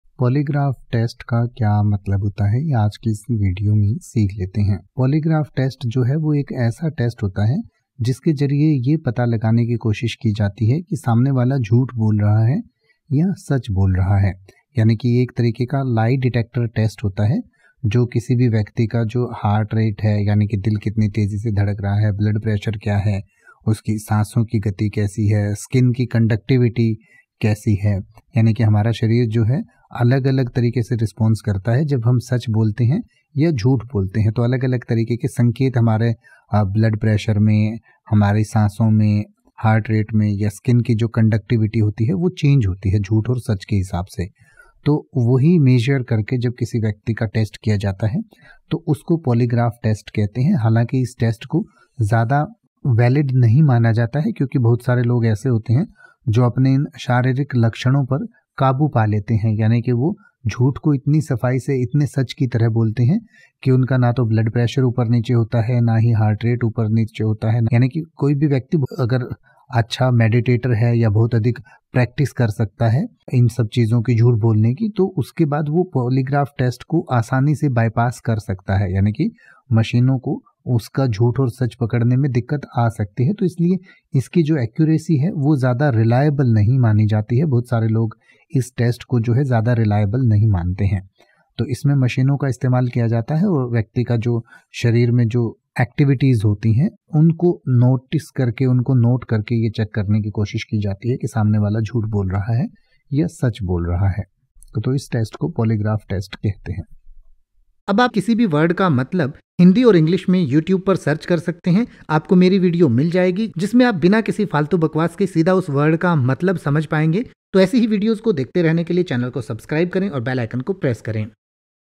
Polygraph test meaning in Hindi | Polygraph test ka matlab kya hota hai | English to hindi
इस वीडियो में आप Polygraph test का हिंदी में मतलब समझेंगे और साथ में Polygraph test का pronunciation भी सीखेंगें यानि की आप Polygraph test के अर्थ के साथ साथ ये भी सीखेंगें की Polygraph test को कैसे बोले या Polygraph test को बोलने का सही तरीका क्या है |

तो बस एक मिनट में सीखिए Polygraph test को|
Let’s learn Hindi Meaning of Polygraph test in detail.

Polygraph test का हिंदी में अर्थ होता है
एक ऐसा टेस्ट है जिसके ज़रिए किसी व्यक्ति के सच बोलने या झूठ बोलने का पता लगाया जाता है

A polygraph test, commonly known as a lie detector test, measures physiological responses such as heart rate, blood pressure, respiration, and skin conductivity while a person answers a series of questions. The underlying theory is that deceptive answers will produce physiological responses that differ from those associated with truthful answers. However, the accuracy and reliability of polygraph tests are widely debated.

Polygraph test को इन उदाहरणों से और अच्छे से समझ सकते हैं